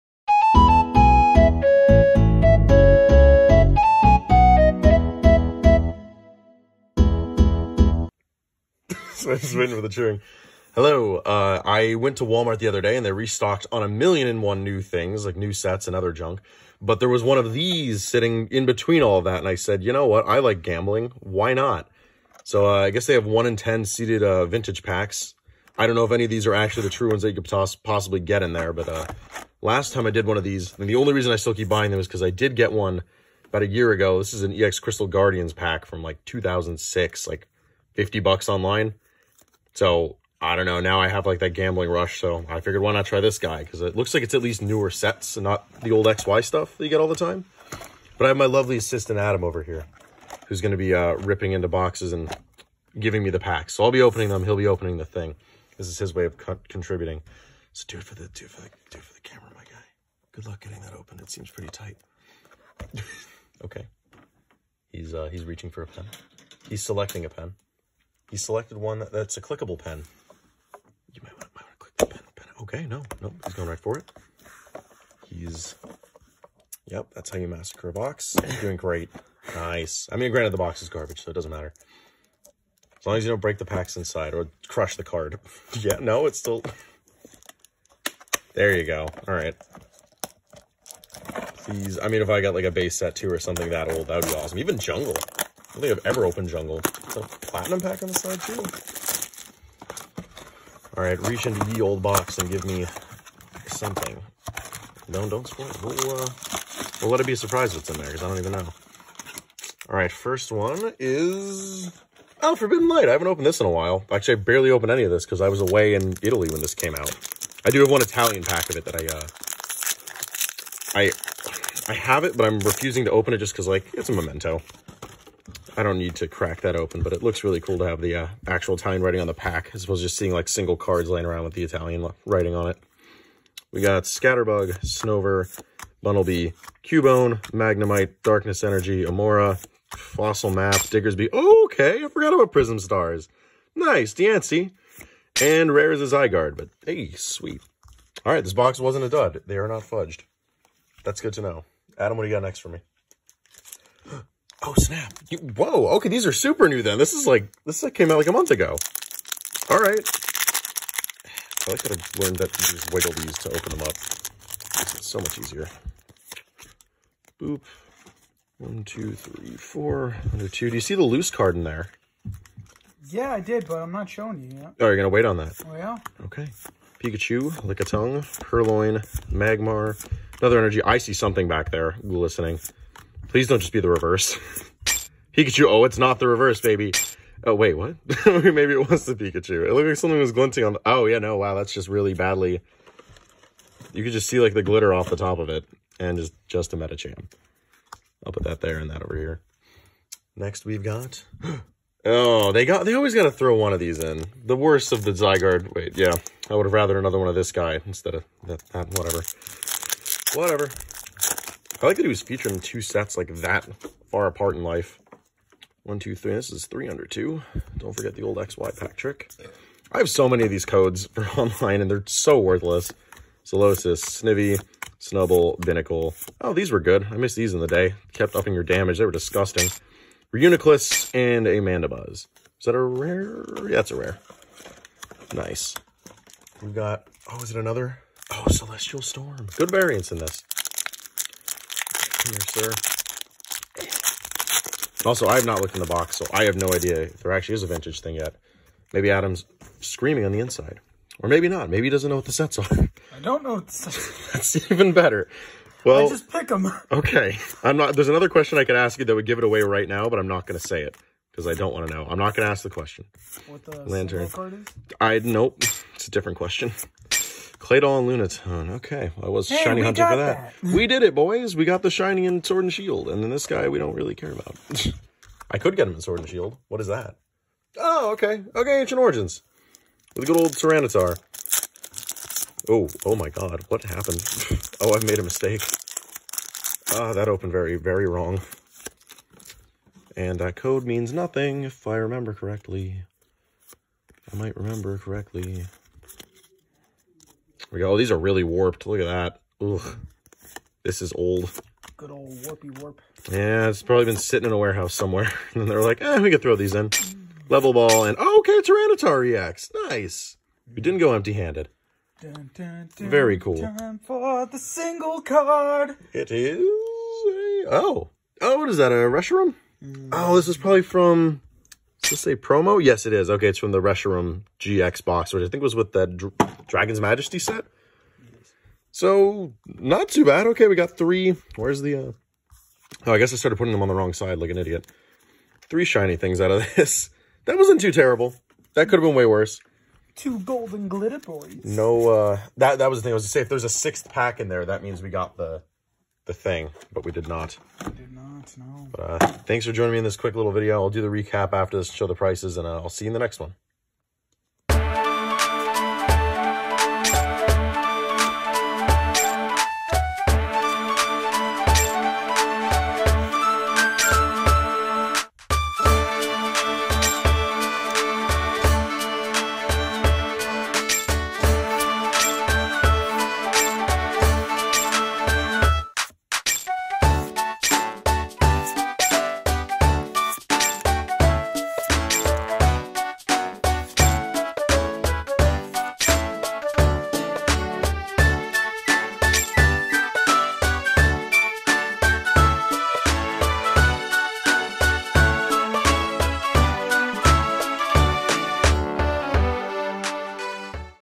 Mystery Power Box (1:10 Vintage Pack Odds) Opening
Watch me gamble my money away on this Christmas released Installment!